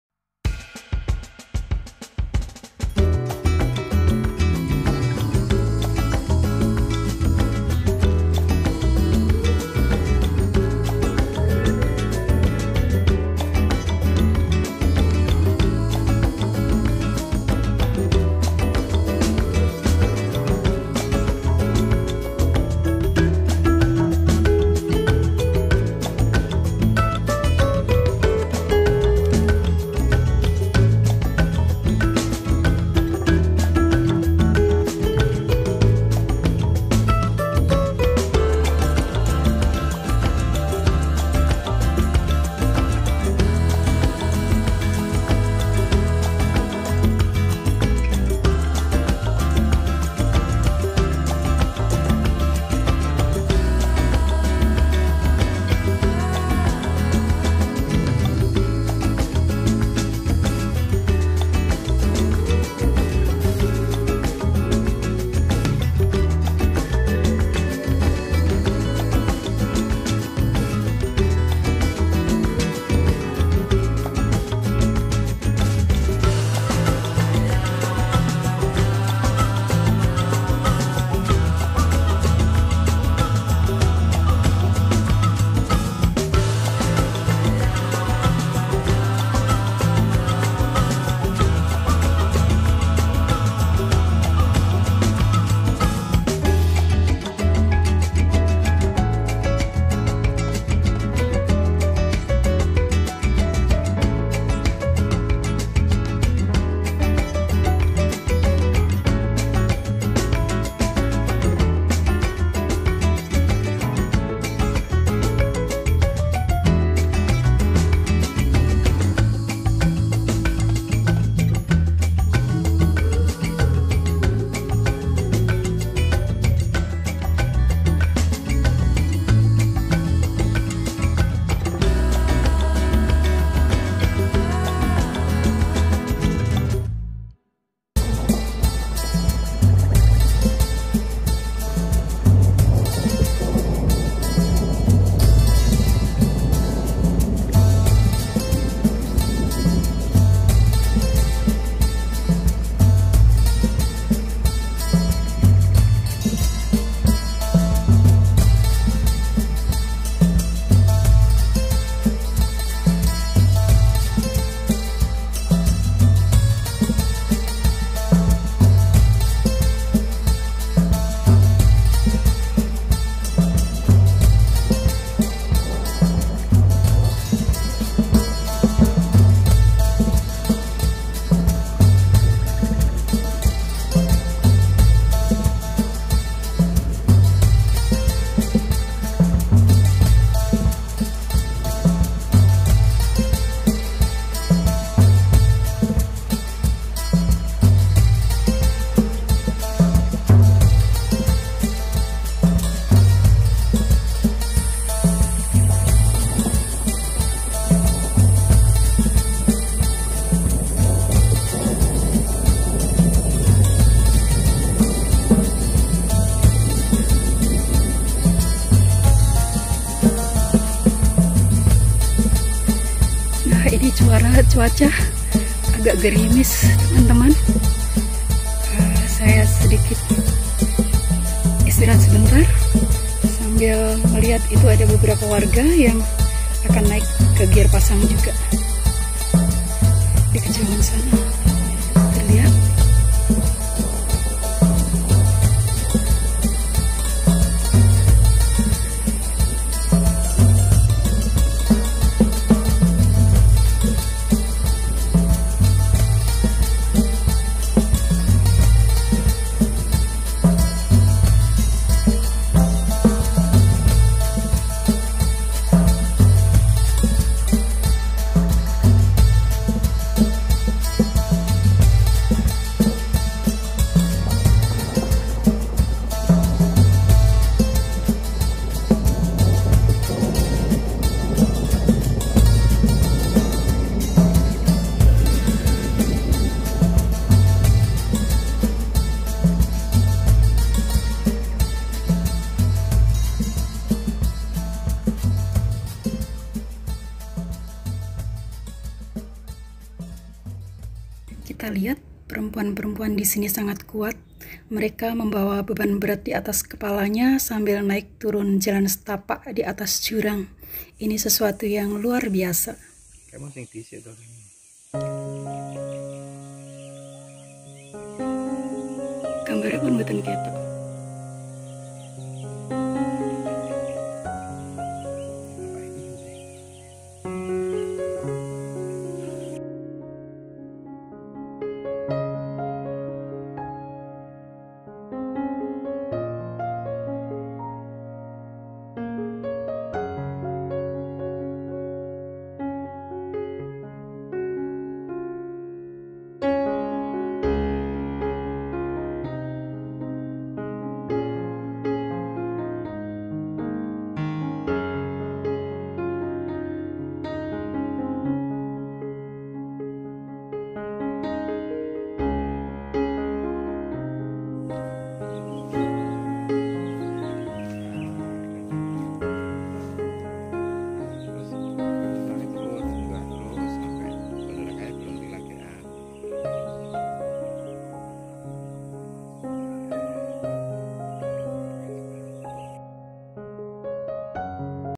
Menerabas jurang ke Girpasang, kampung terisolir Gunung Merapi
Kampung unik Girpasang di lereng Gunung Merapi, Desa Tegalmulyo, Kecamatan Kemalang, Kabupaten Klaten, Jawa Tengah, merupakan kampung terisolir.
Bagi kalian yang menyukai wisata ekstrim, silahkan tonton video ini sampai selesai.
Jangan lupa SUBSRIBE dan tekan tanda loncengnya agar mendapatkan video-video pesona alam dan kearifan budaya dari channel Sri Kaer.

Di kampung ini hanya ada 9 rumah yang dihuni 12 KK terdiri 34 jiwa. Dikelilingi jurang berkedalaman sekitar 130 m. Jalan setapak diatas jurang menjadi akses utama ke kampung ini.
Meski medanya berat, Girpasang memiliki pesona alam yang luar biasa.